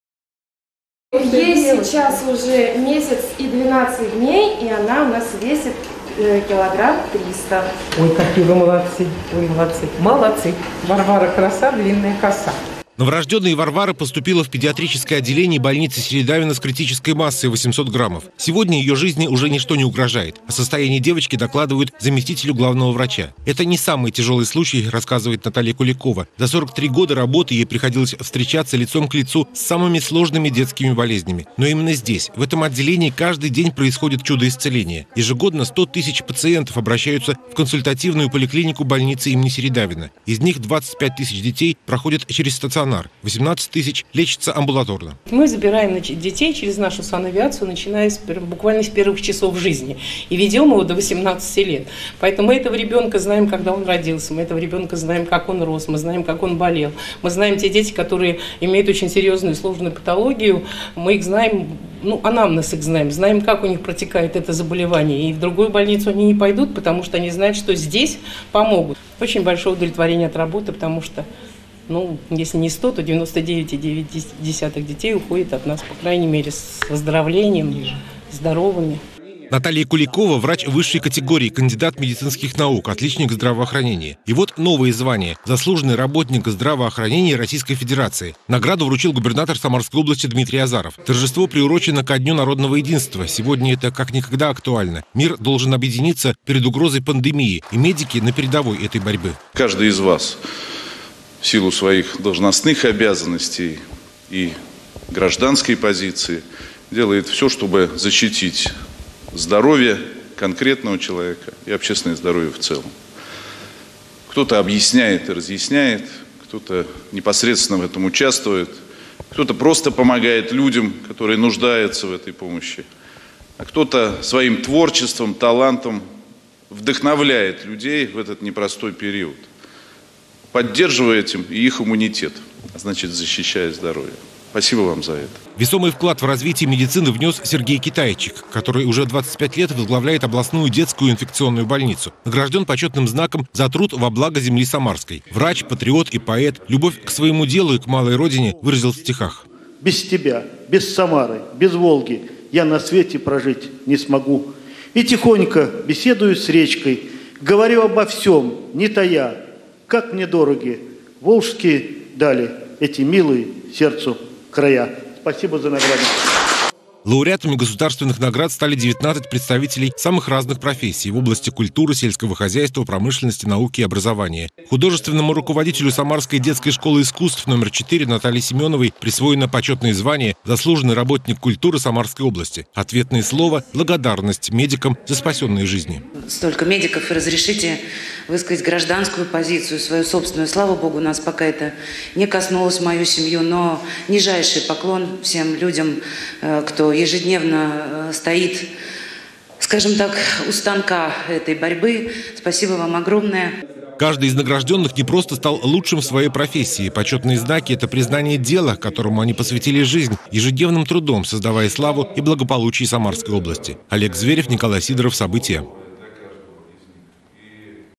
Девятнадцать жителей Самарской области стали лауреатами государственных наград
Девятнадцать жителей Самарской области стали лауреатами государственных наград. Почетные знаки и дипломы вручил губернатор Дмитрий Азаров на торжественном собрании, посвященном Дню народного единства. В этом году особая благодарность - медикам, которые сейчас стоят на передовой борьбы с коронавирусной пандемией. Впрочем, награды получили и представители других профессий, которые обеспечивают благополучие жителей региона и страны.